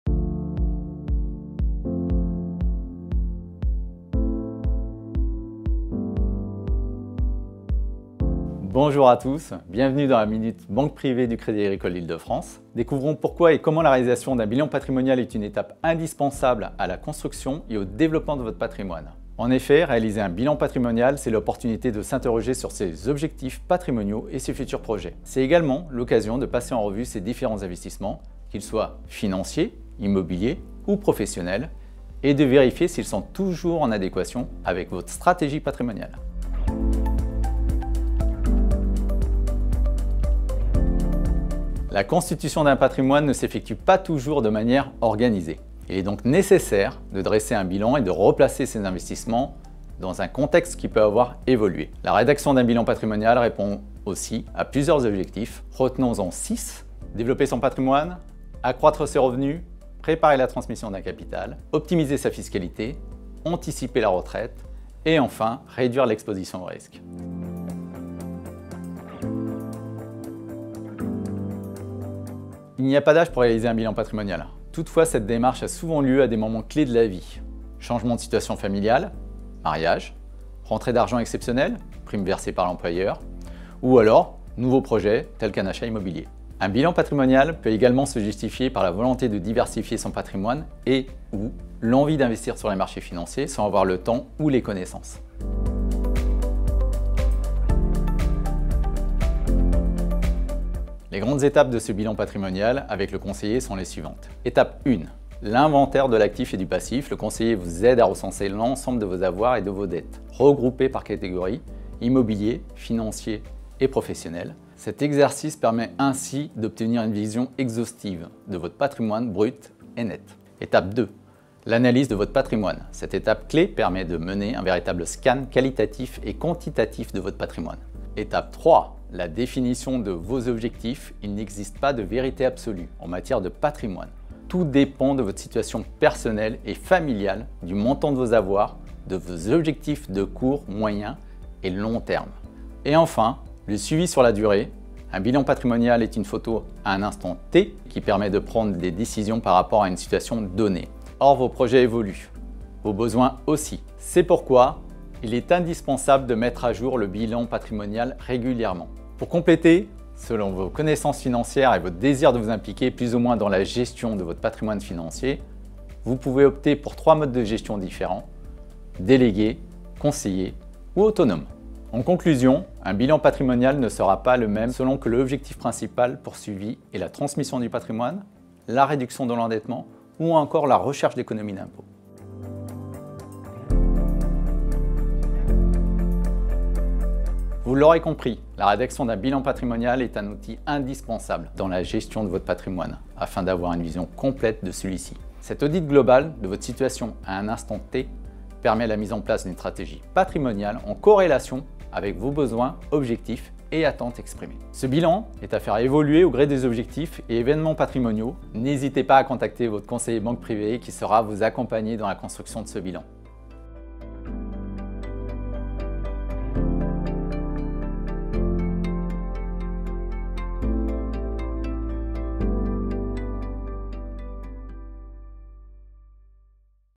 Minute Banque Privée : Pourquoi et comment réaliser un bilan patrimonial ?
Réaliser un bilan patrimonial, c’est l’opportunité de s’interroger sur ses objectifs patrimoniaux et ses futurs projets. C’est également l’occasion de passer en revue ses différents investissements et de vérifier s’ils sont toujours en adéquation avec sa stratégie patrimoniale.
David Dos Santos, conseiller privé revient sur cette étape indispensable pour la construction et le développement d’un patrimoine.